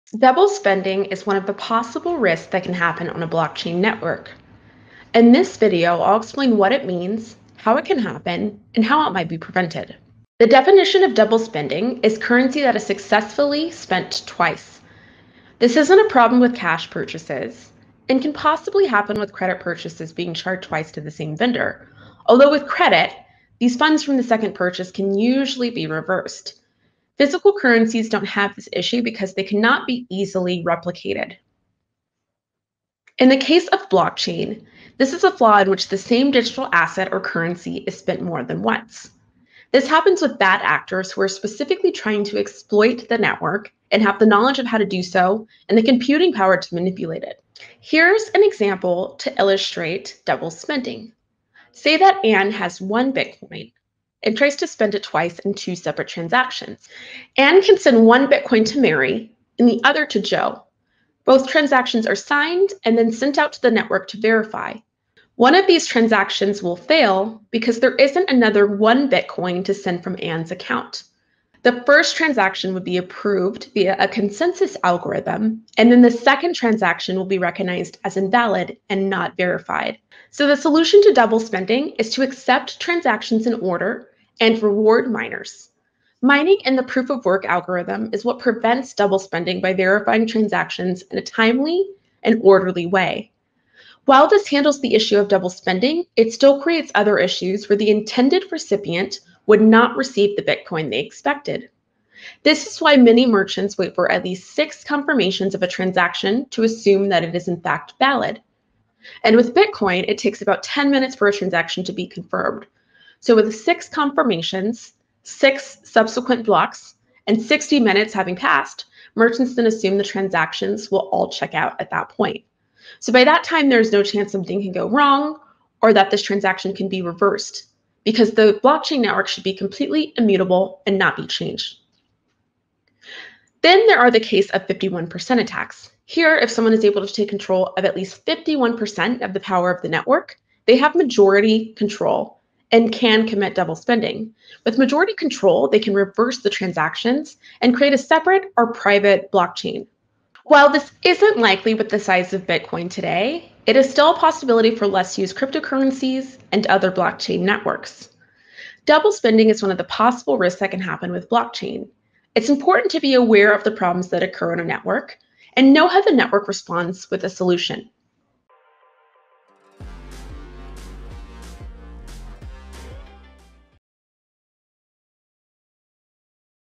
Double spending [13 of 20] | Blockchain for Beginners
Each blockchain has a series of nodes which are all connected. Some nodes store copies of the entire blockchain, some are for transaction verification, and some are for mining. Learn about the different types of nodes and how they all work together.